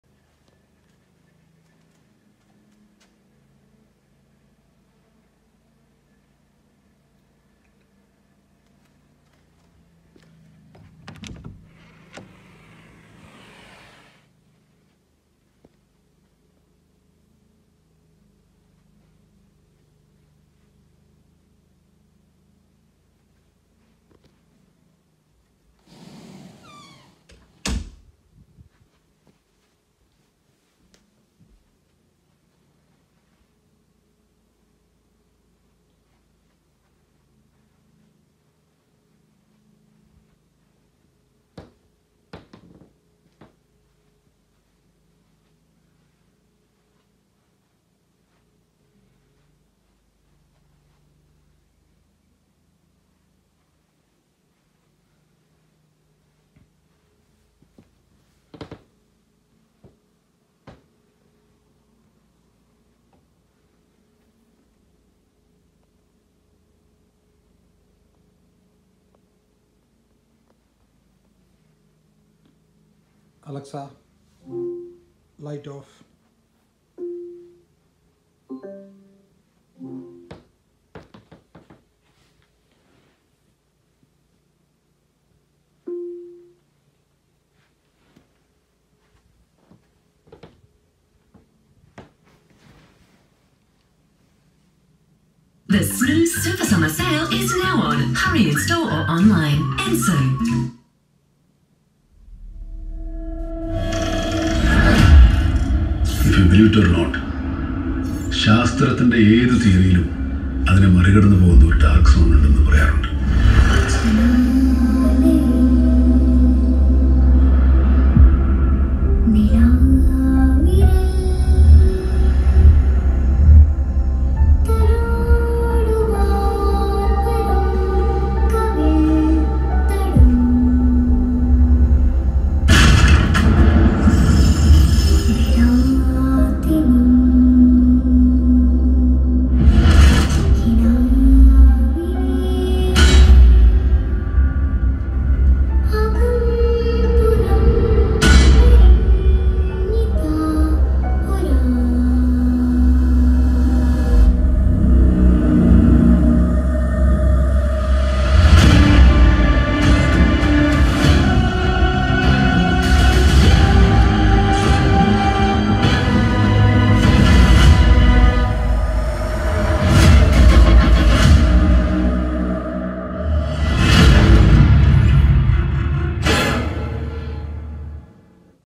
160” cinemascope (Anomorphic) 2.39:1 Home Theater + Epson TW9400
Home theater screen
- Encore cinevue 160” cinemascope
Audio- Bose Lifestyle 650 with dual acoustimass 300 bass module
Projector- Epson TW9400
Media player- Nvidia shield tv pro